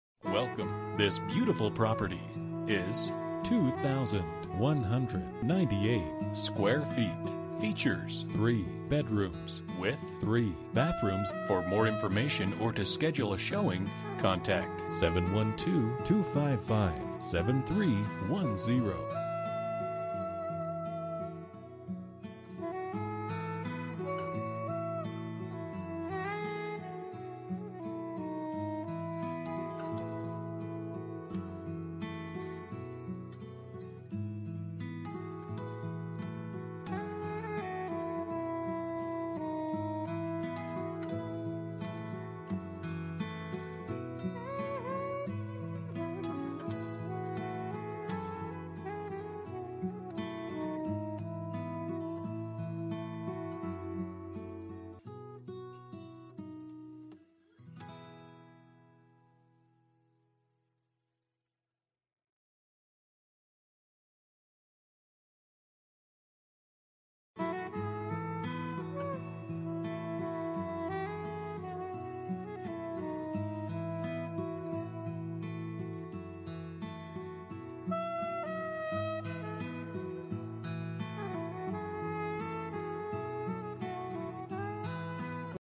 Property for sale - 1401 Windham St., Sergeant Bluff, IA 51054
Bedrooms: 3
Bathrooms: 3.5 bathrooms
Square Feet: 2198
Price: $270,000

For more information about this property, please contact Jean Midland at or . You can also text 3326588 to 67299.

Coldwellbankerabr.com
03/08/2016 03:53:02 am

Last modified: